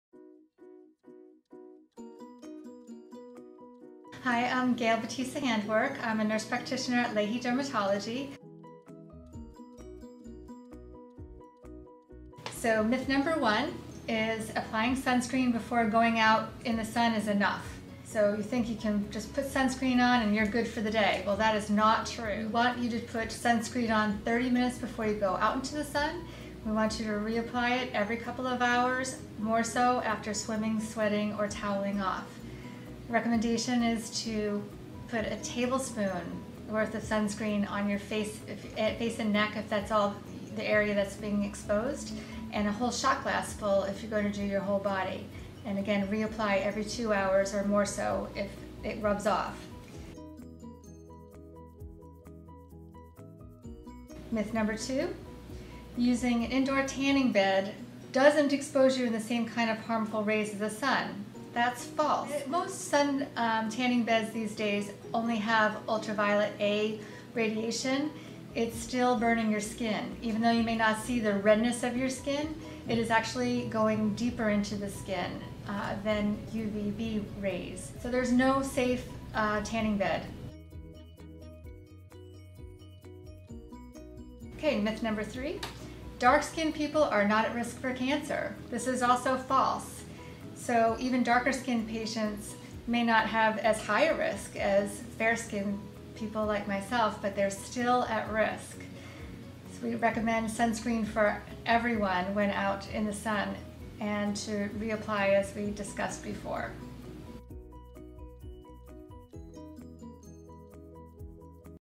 Thursday Three: Skin Care Myths
Love spending time in the sun? To help keep your skin healthy, Gail Batissa Handwork, DCNP discusses misconceptions around applying sunscreen, tanning beds and risks for skin cancer in this week’s ThursdayThree.